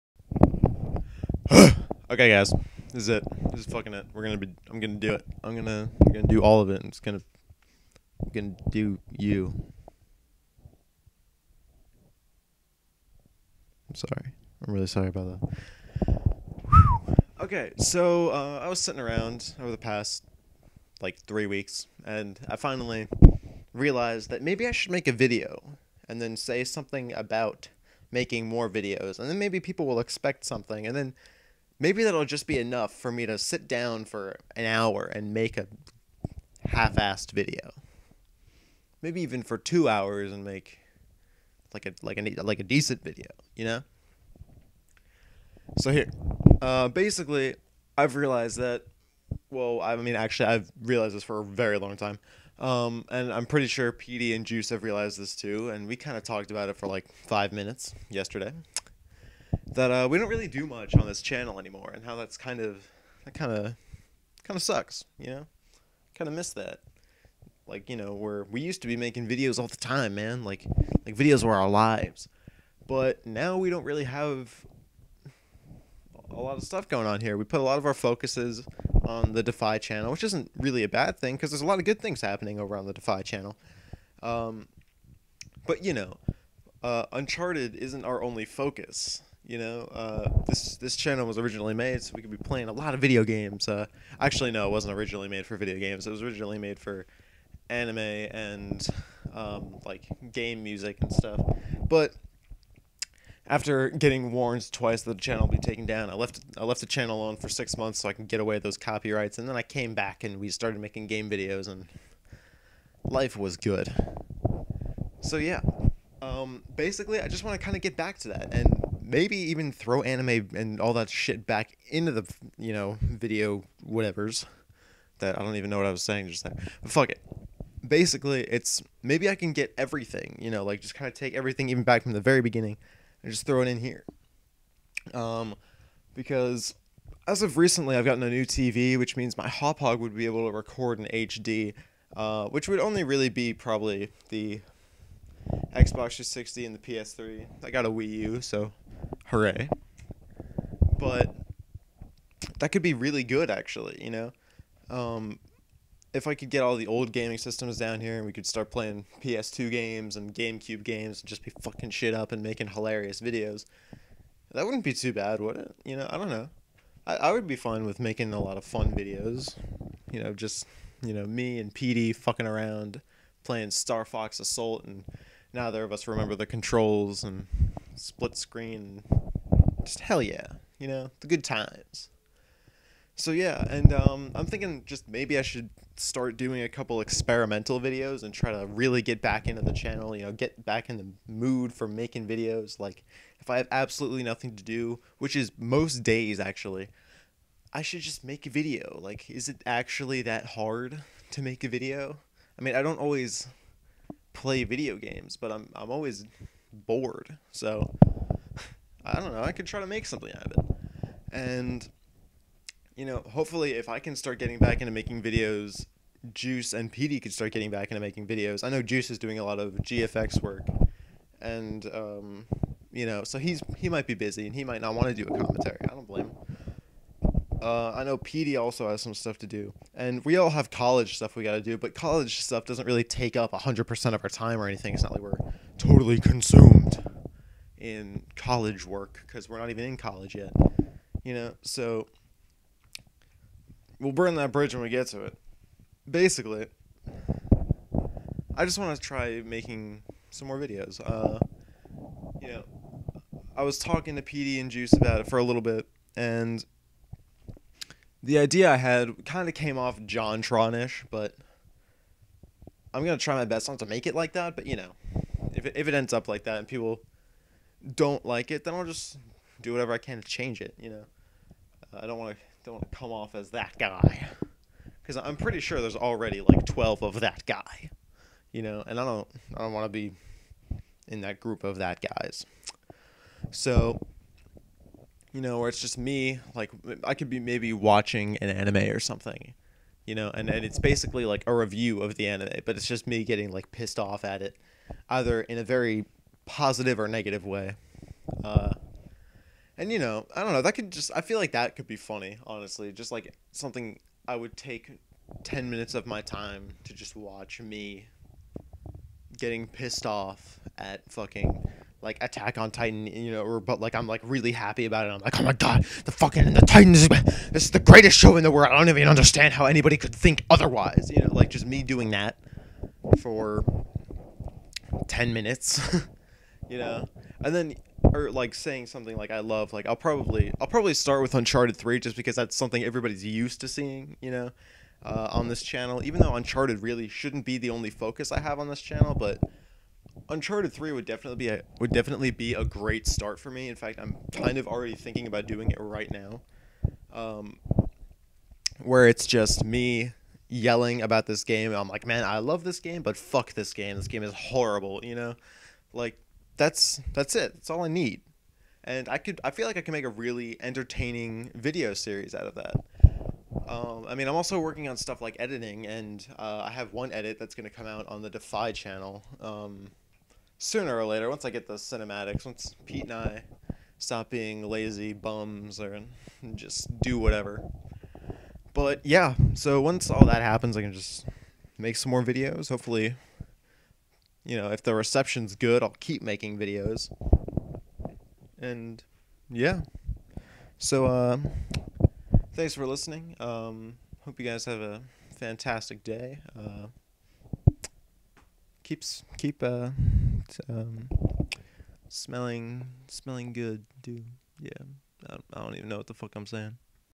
Video With Relevant Title
Basically, we're getting some shit done and can hopefully bring out some awesome videos for you guys within the next few weeks. It all comes down to our laziness, and once we get past that, nothing's going to stop us. Right now we're going to start with mostly gaming videos, most likely on PS3 and PS4, but if we can figure out a way to make it work, I'll definitely be playing Wii U games and Xbox games. Really, we have a lot of fun things we're thinking about, and it's just a matter of getting all done. Hope you guys enjoy! Comment on what games you want to see, and we might just play it and make some videos!